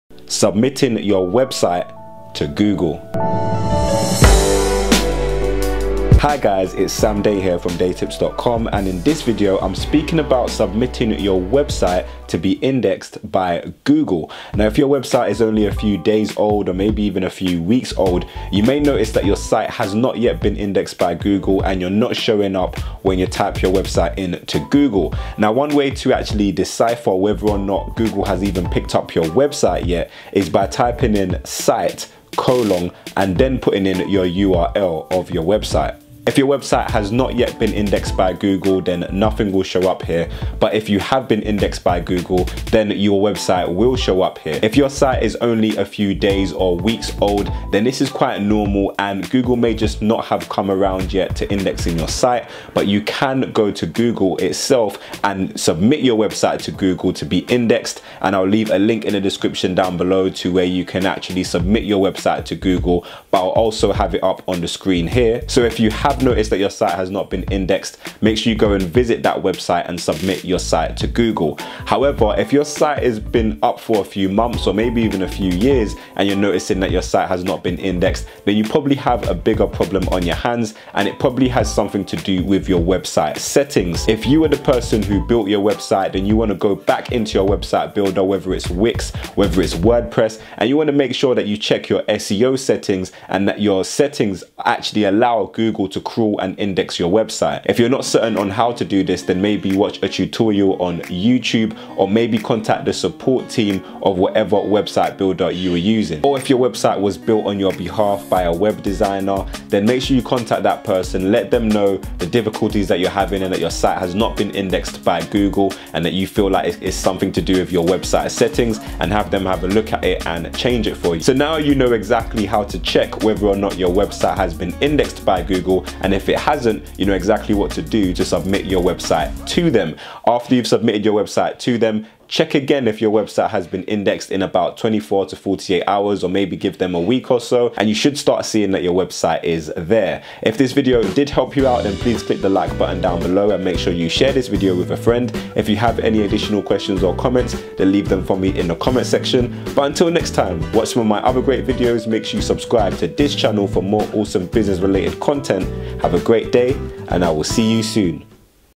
Submit url to Google : How to Submit a Site to Google
Click here to discover how to drive sales to your products or services online with one simple formula

Submit url to Google Tutorial

In this video I am teaching how to submit url to google with ease and simplicity. Submitting a url to google is really easy, however you should first check if your site has already been indexed by Google. by typing in site:yoursite.com and searching that on google. If you find that your site is already there, you do not need to submit your site to google. However if your site is not there then I suggest you do. If your site is not there and your site has been published for over 2 or 3 months, there may be a deeper issue than just submitting your site to google.